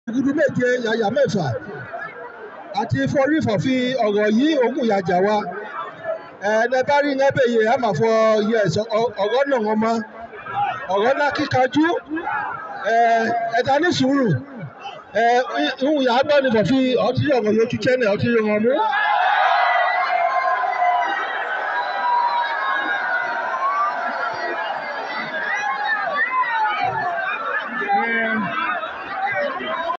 Suspected Killers Of Owo Catholic Parishioners Arrested